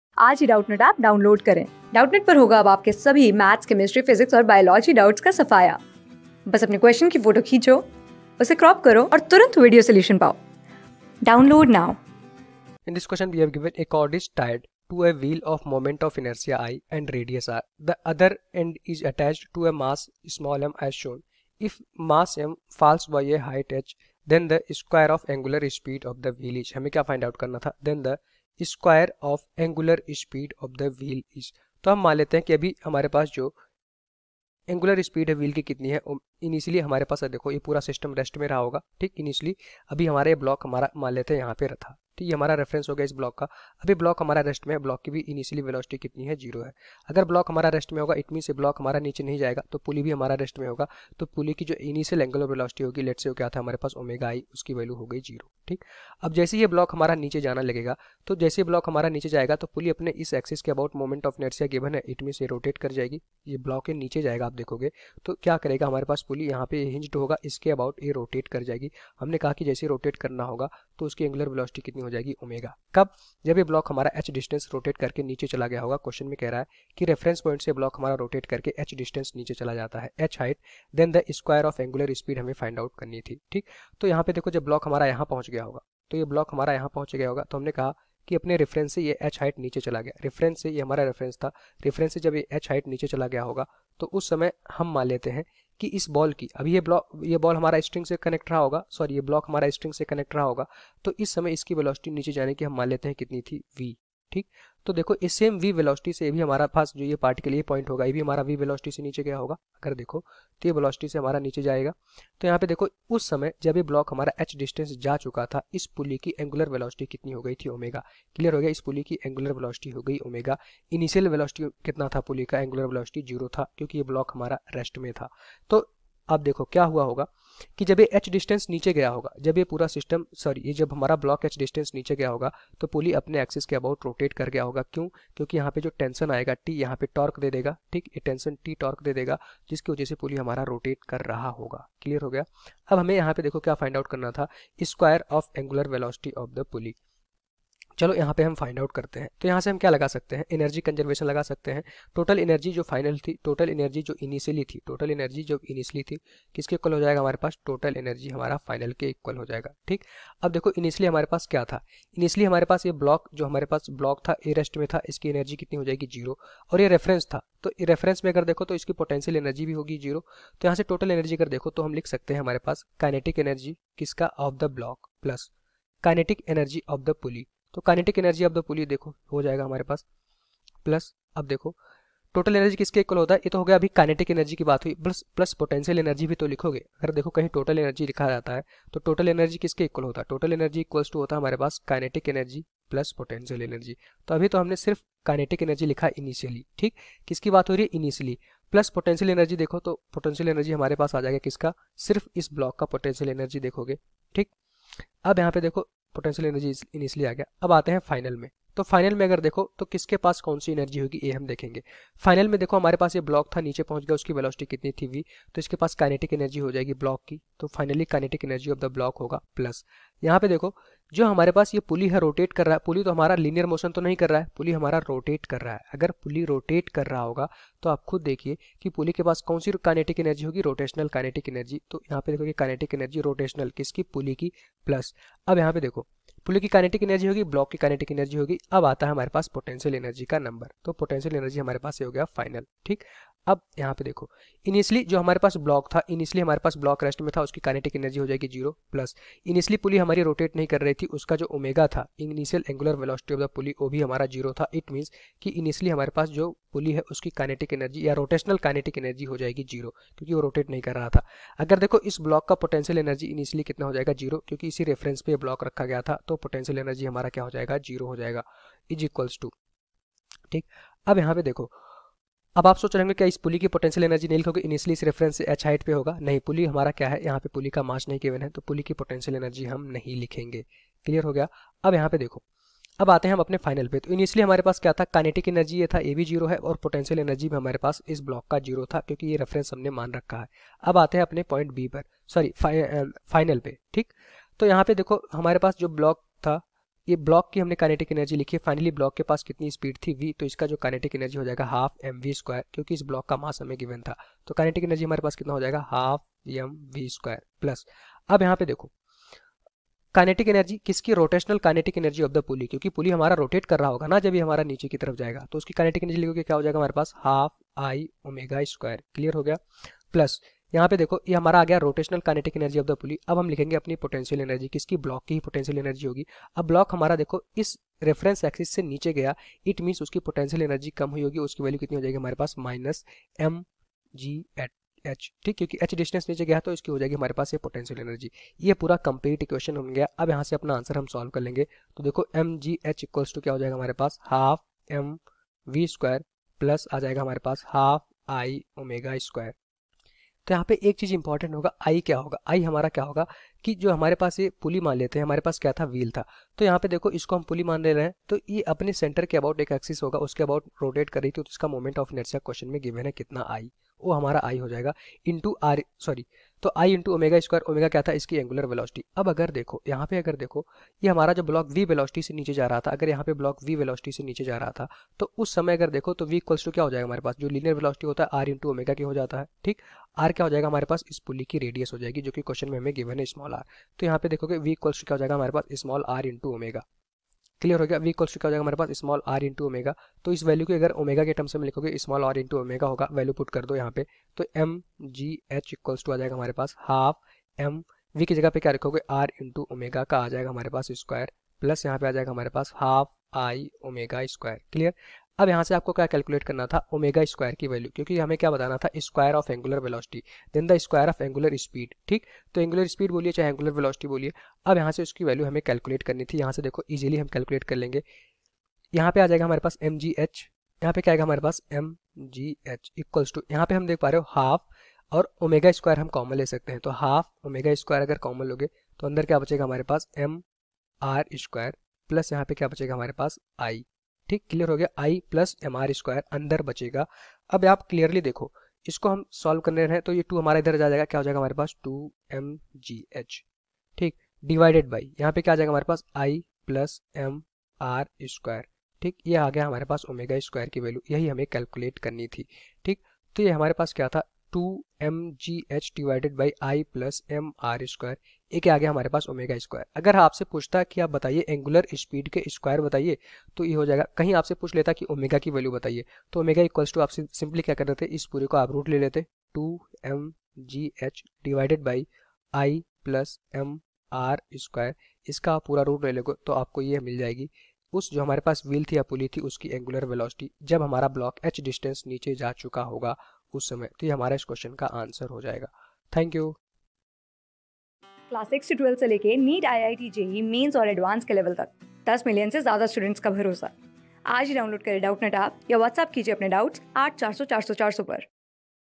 A chord is tied to a wheel of moment of inertia I and radius r. The other end is attached to a m...
A chord is tied to a wheel of moment of inertia I and radius r. The other end is attached to a mass m as shown. If the mass m falls by a height h then the square of angular speed of the wheel is ?
Class: 12
Subject: PHYSICS
Chapter: JEE MAIN 2021
Board:IIT JEE

👉 Have Any Query? Ask Us.